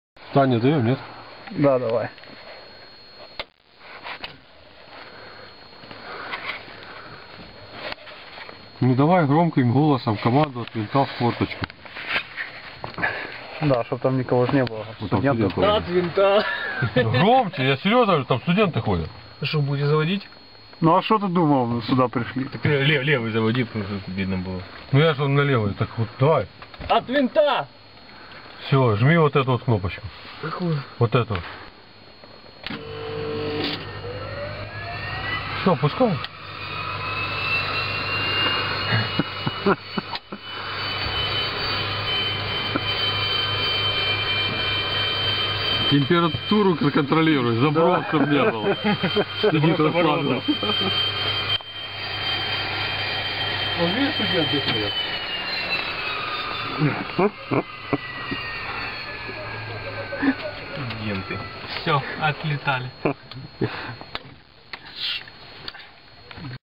L 410
Холодная прокрутка двигателя Л-410. Киев 2008.